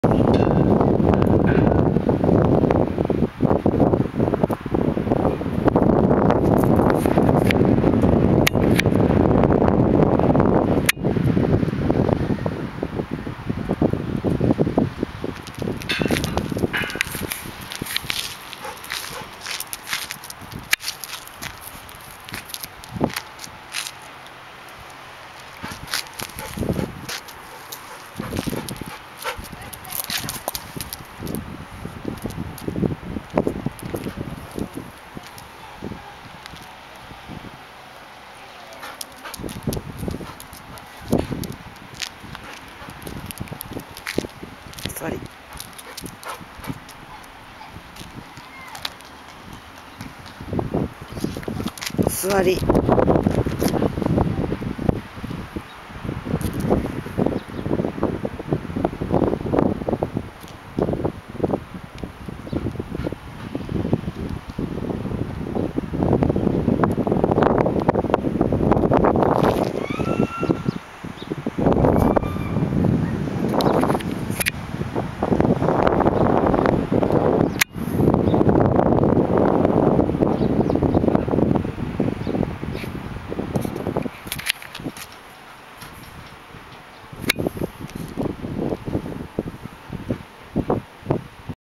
何故か揺れてるそうです 2010-04-26
公園に立てられてるポール。いくつもあるんですが　これだけ
いつも揺れてるそうです。
ちょっとこわいですね。
その音にビビッてる夢丸です。